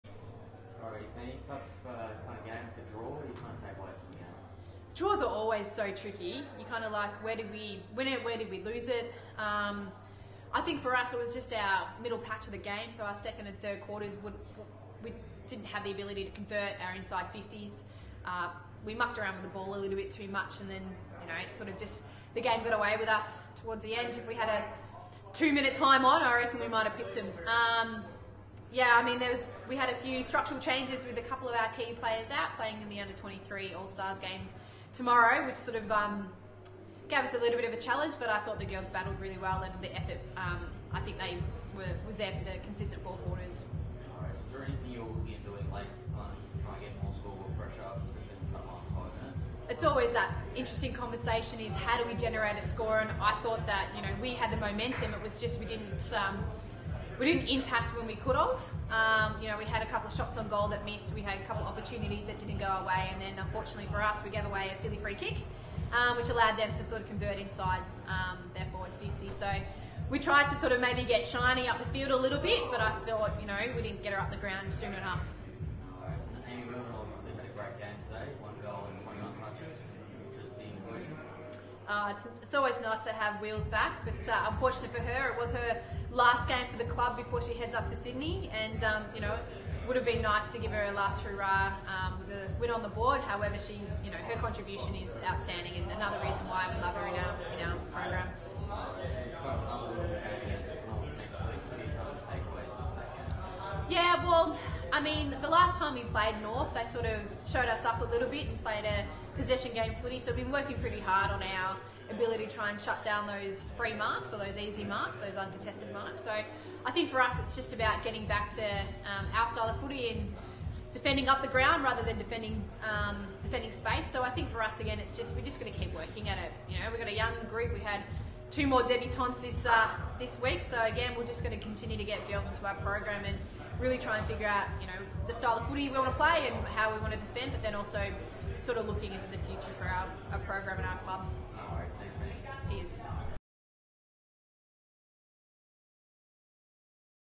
Penny Cula-Reid Post-Match Interview | VFW Round 11
Hear from VFLW's Senior Coach Penny Cula-Reid after Saturday's draw against Carlton.